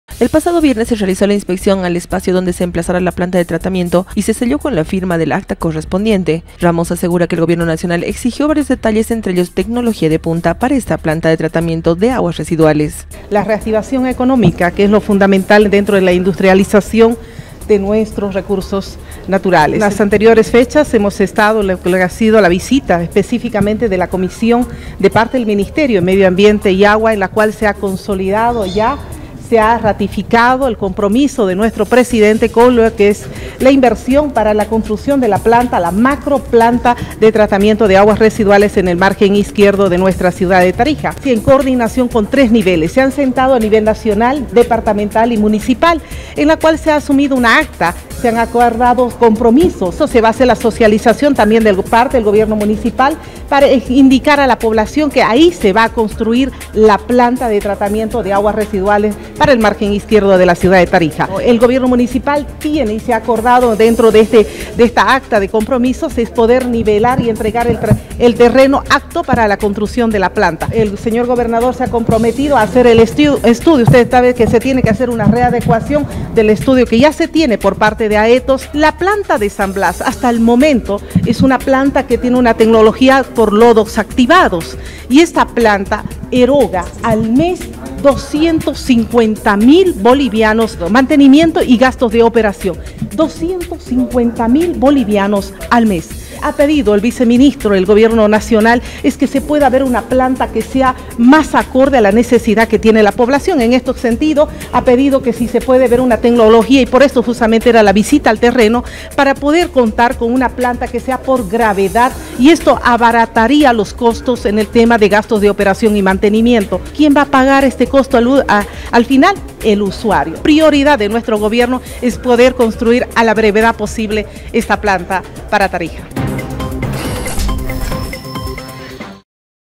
CON FIRMA DEL ACTA DEL GOBIERNO, GOBERNACIÓN Y ALCALDÍA
Luego de la inspección realizada a los terrenos el pasado viernes y con la coordinación de la gobernación y alcaldía en la ruta crítica, la concejal Ramos, asegura que existe la predisposición del Gobierno nacional, pero exige que se haga en un lugar donde tenga pendiente y además que se construya con tecnología de punta.